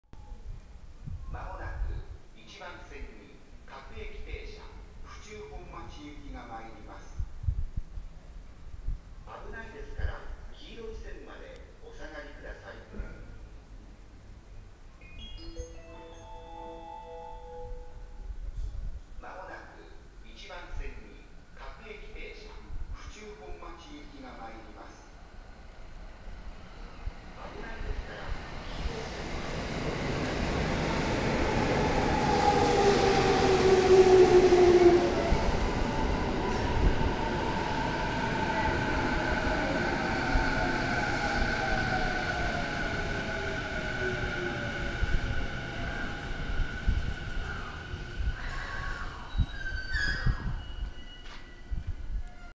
武蔵野線209系各駅停車府中本町行を撮った。新秋津駅
新秋津駅にて